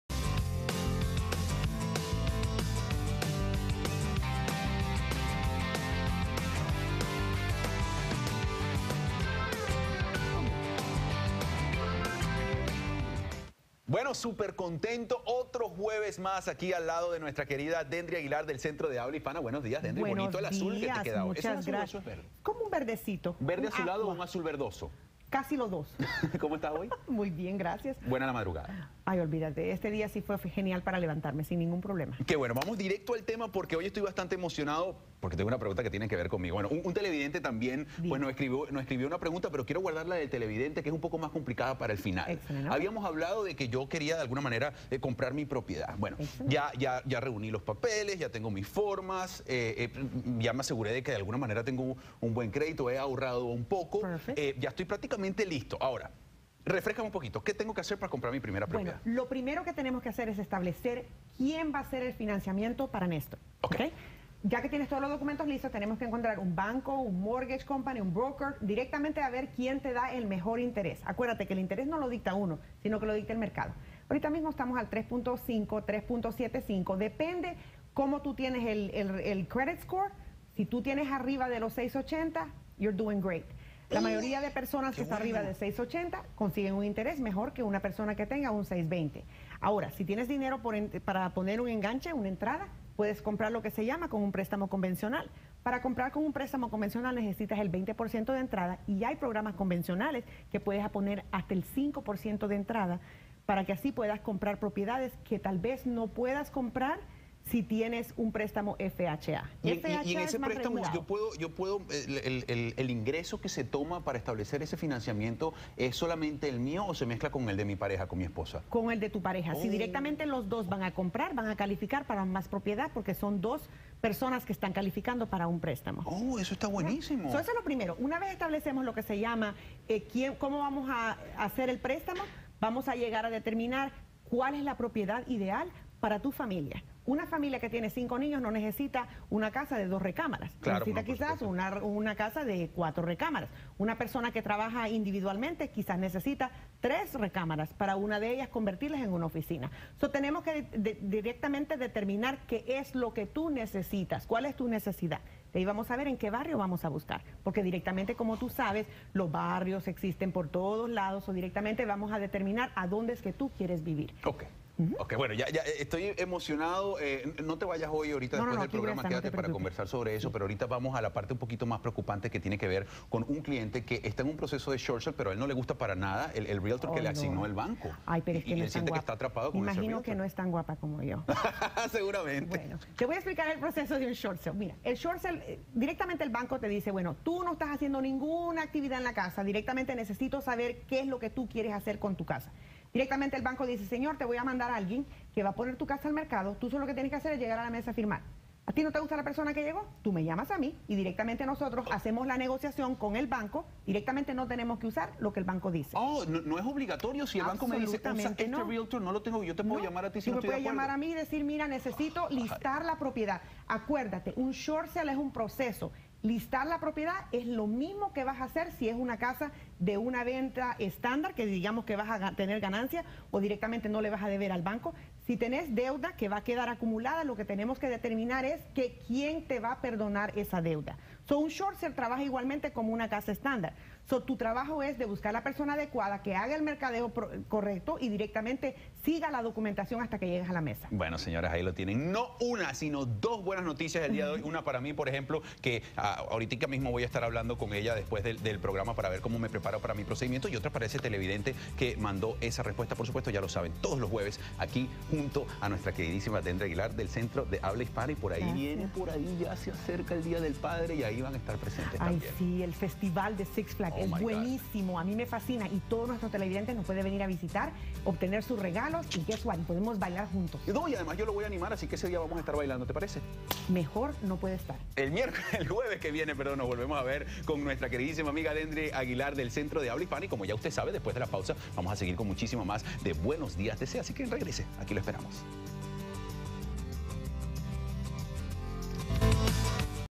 Nestor quiere comprar una casa
Washington D.C.- Dendry Aguilar del CHH recomienda como prepararse para comprar una casa por primera vez. Además, responde las preguntas de los televidentes.